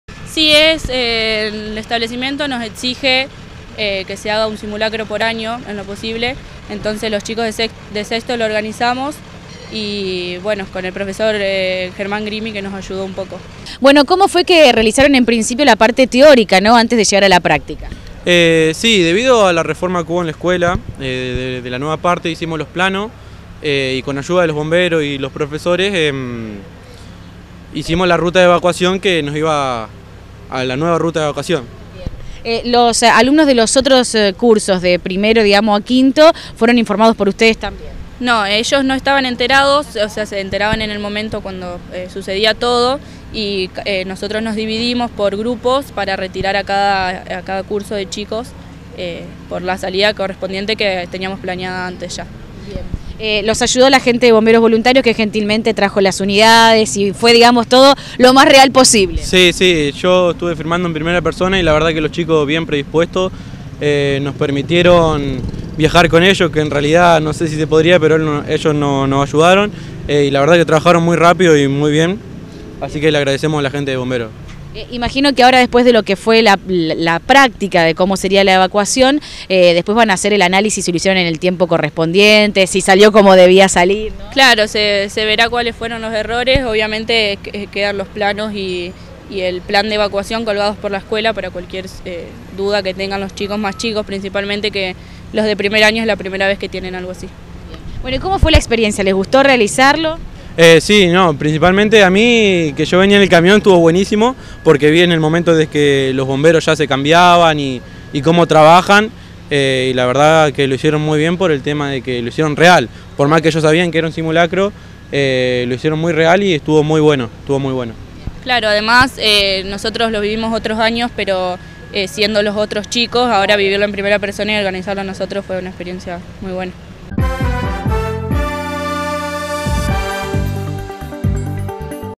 Camila - Luciano - Alumnos Escuela Técnica Nº631
Realizaron un simulacro de evacuación en la institución escolar